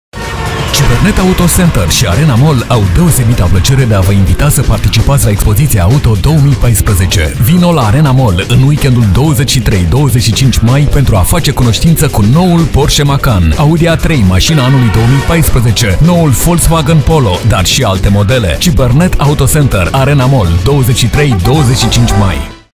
Cybernet Auto Center - Arena Mall Auto Expo 23-25 Mai 2014
Cybernet Auto Center si Arena Mall au deosebita placere de a va invita sa participati la Expozitia Auto 2014.
Vino la Arena Mall in weekendul 23-25 Mai, pentru a face cunostinta cu Noul Porsche Macan, Audi A3: Masina Anului 2014, Noul Volkswagen Polo, Bentley Continental dar si alte
modele.

Te asteptam pentru a testa cele mai noi modele dar si cu alte surprize!

Cybernet Auto Center - Arena Mall 23-25 Mai.